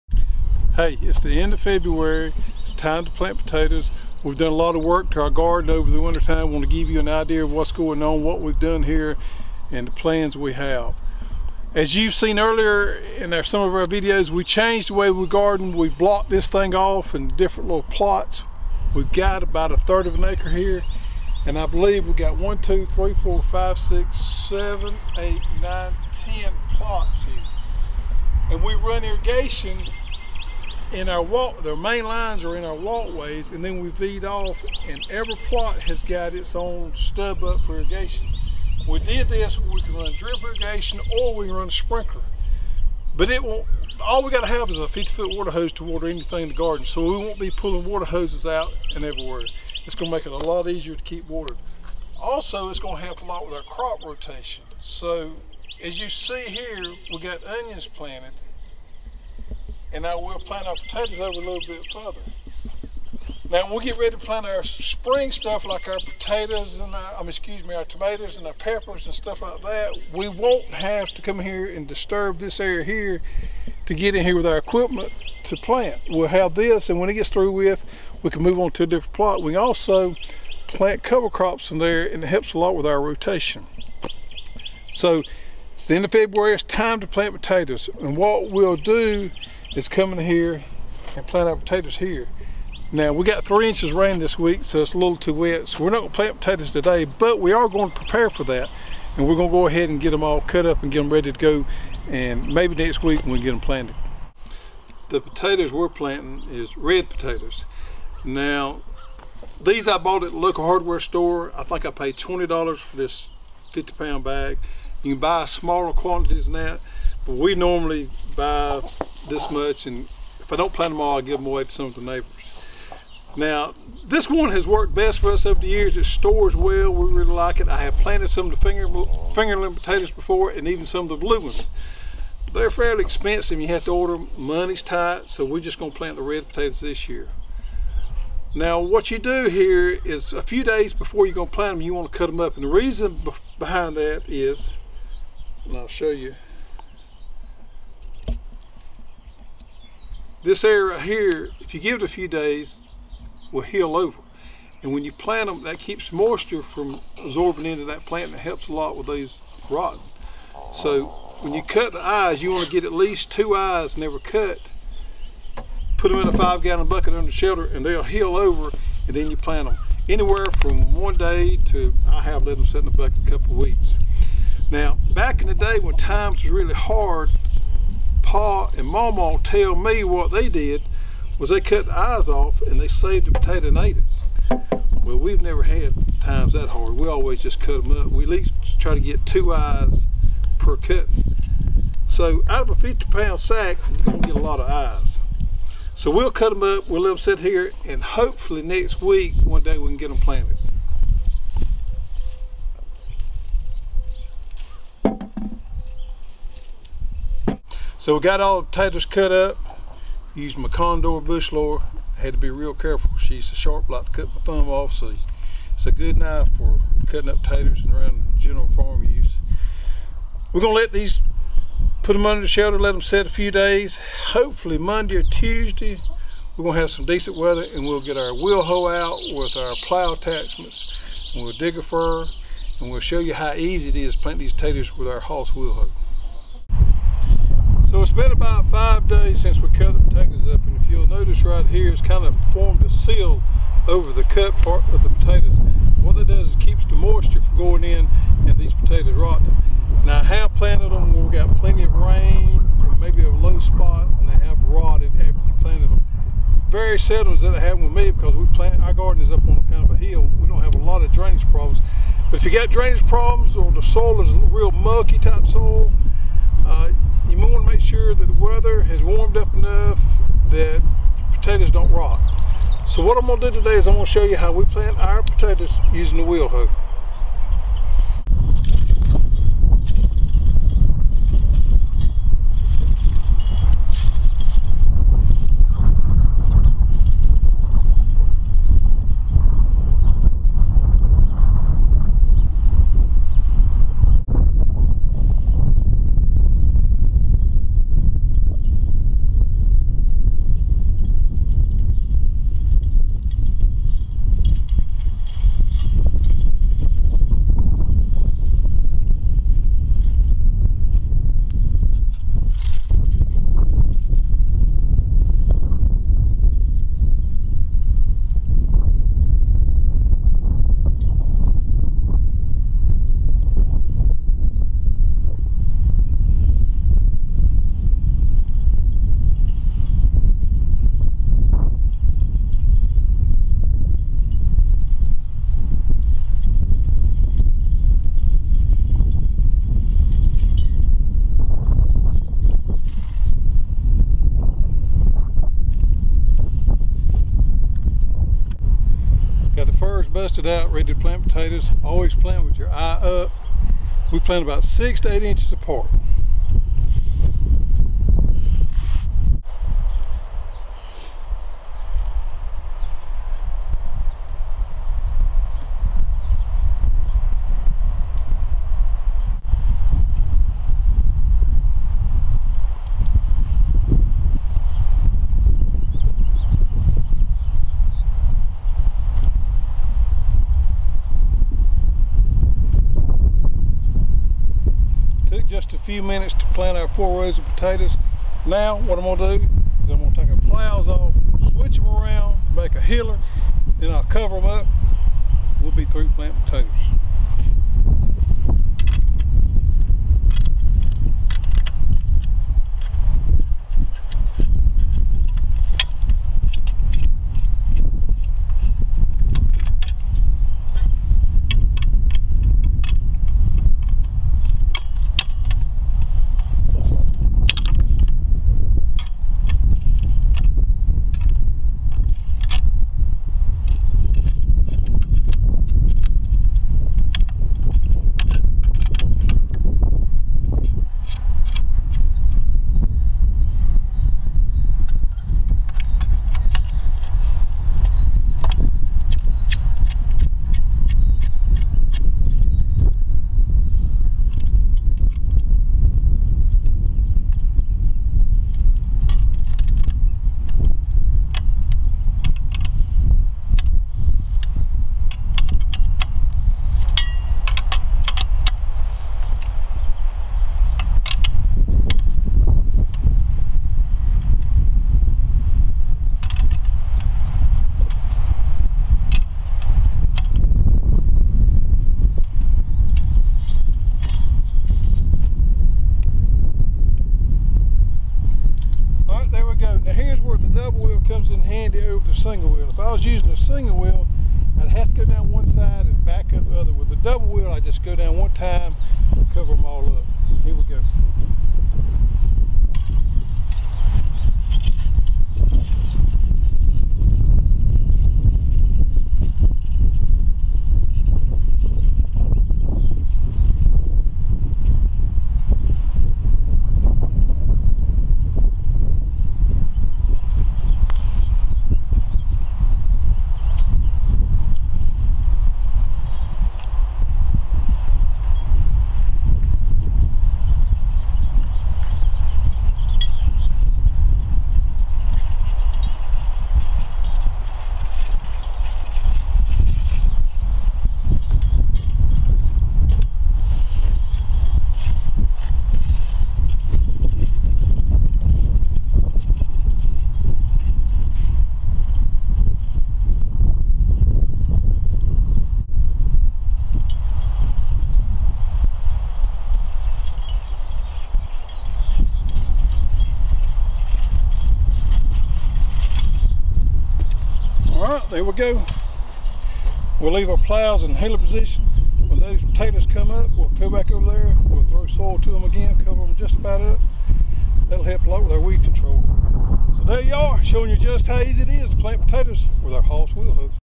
Planting Red 'Taters'
Here we show how we cut up seed potatoes for planting in the Hoss Vegetable Garden.  We planted our early spring crop of Red Potatoes using the Hoss Double Wheel Hoe with Plow Attachment.  We used the Plows to dig a furrow and then turned the plows around to create a hiller to cover the potatoes.